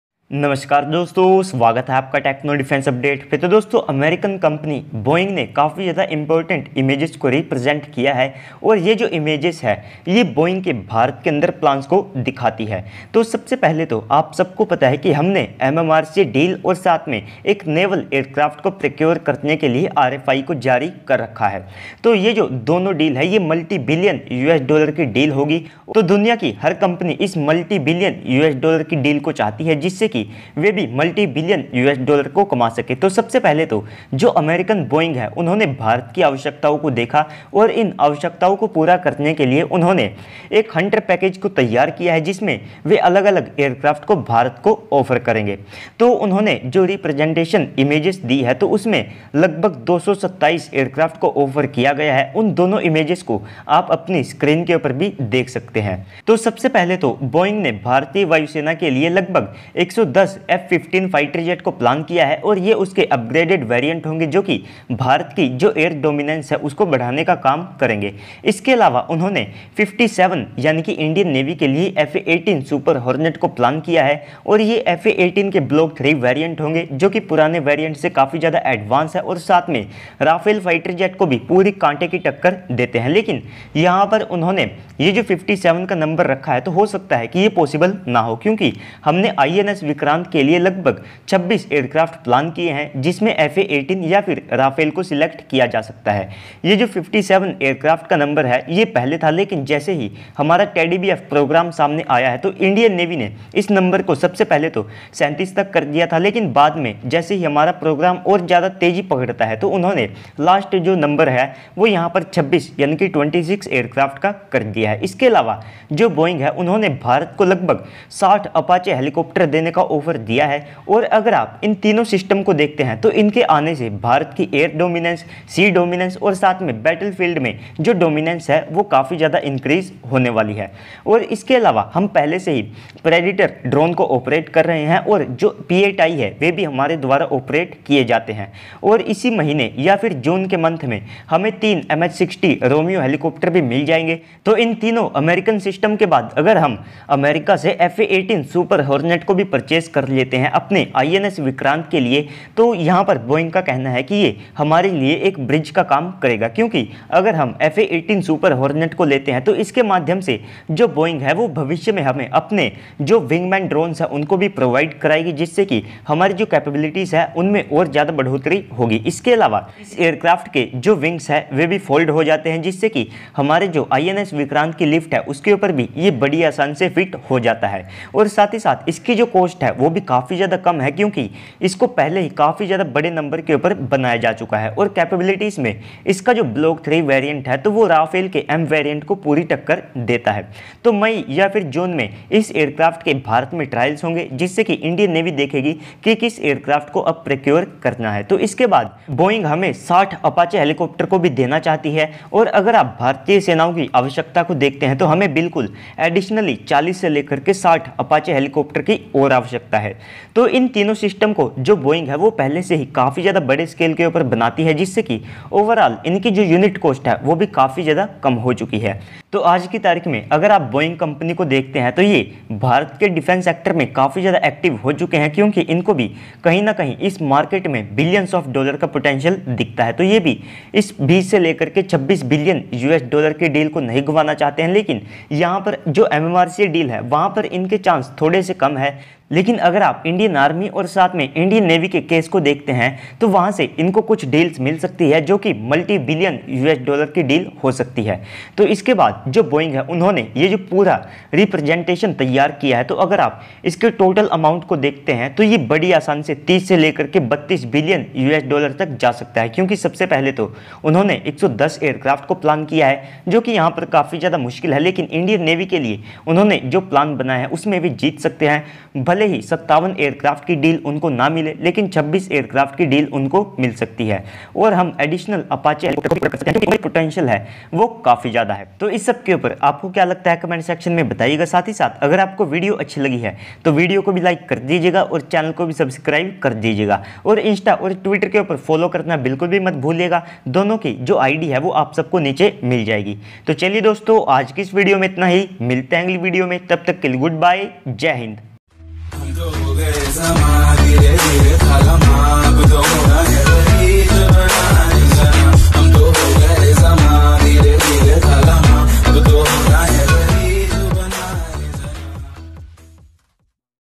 बोइंग ने किया भारत के लिए 227 एयरक्राफ्ट का सबसे बड़ा ऐलान|Fa-18,F-15Ex And Apache To India|Fa-18 IN
For Businesses Enquiries Cantact At

1. Hindi Defense update
2. Daily Economic News
3. International Politics News
4. World News
5. Daily Weapons News & Videos